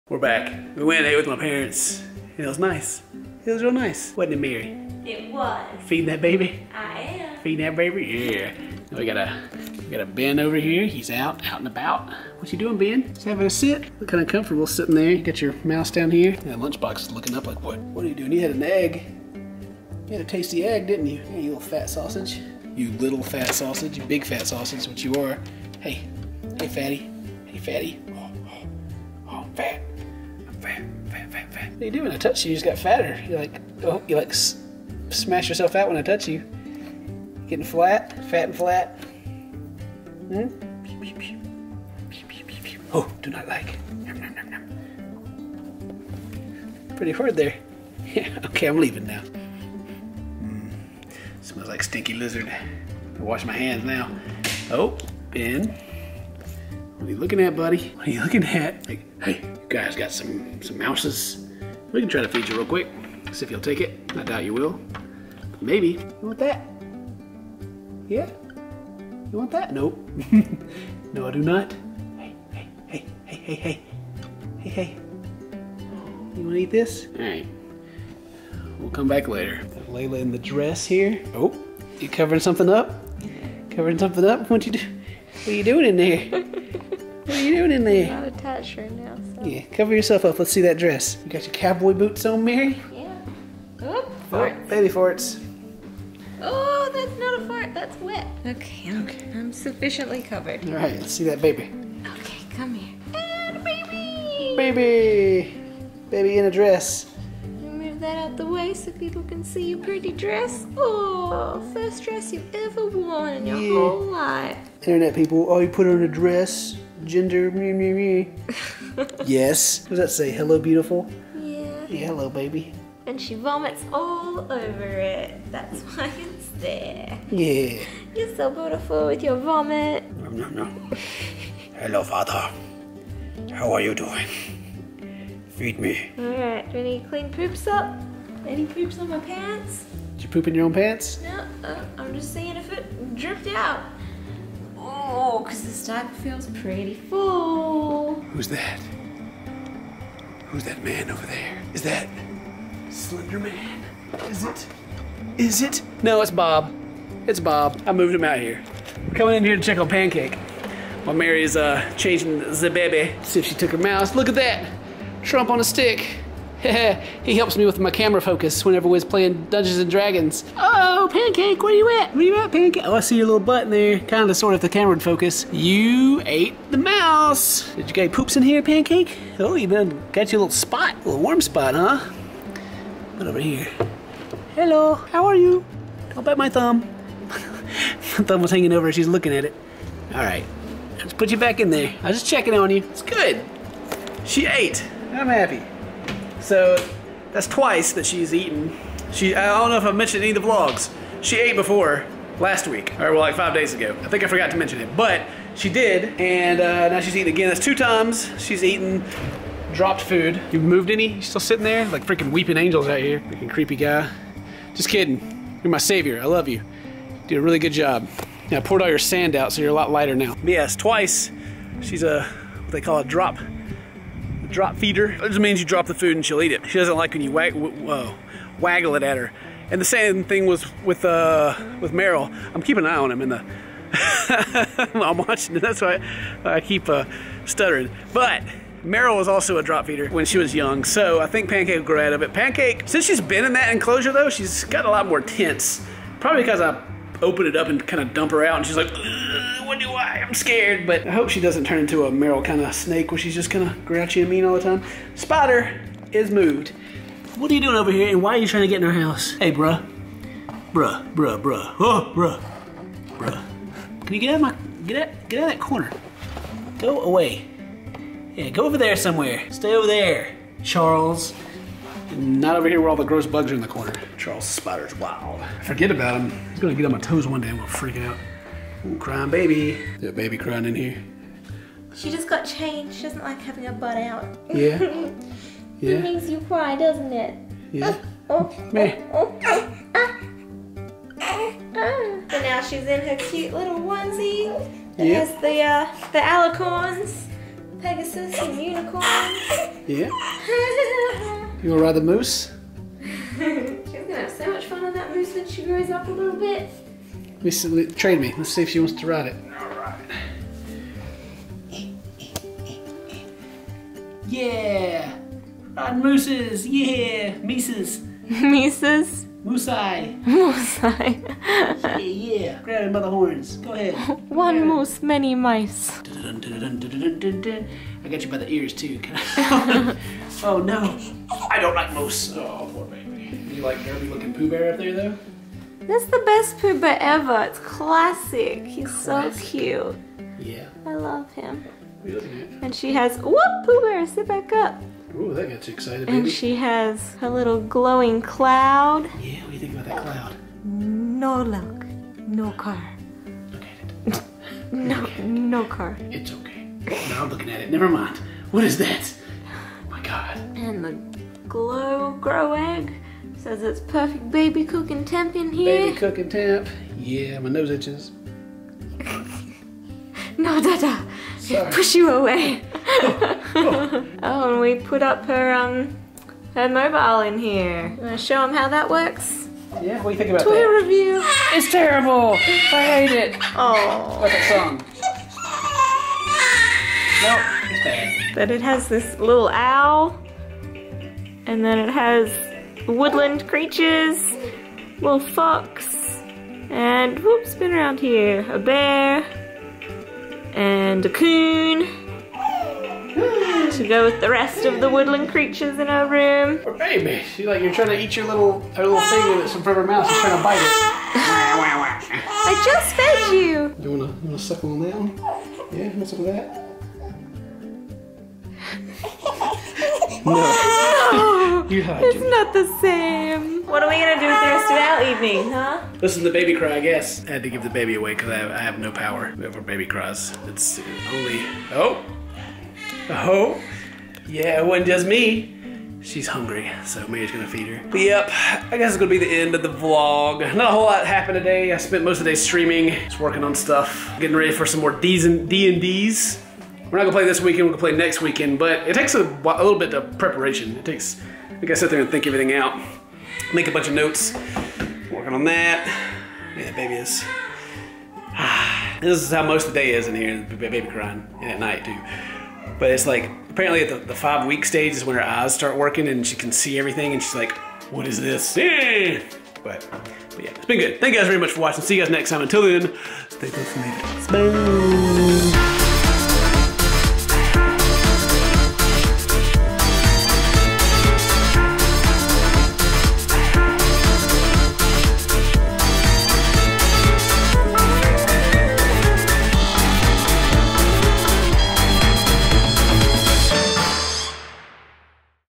BABY'S FIRST DRESS! | Amazing Little Nugget | Family Baby Vlogs
Today we go see my parents and baby Layla wears her first dress! Yaaay! She's so precious. She's also starting to be able to see and the world seems a little spooky. She cries a lot more now. Poor baby.

ʕノ•ᴥ•ʔノ ︵ ┻━┻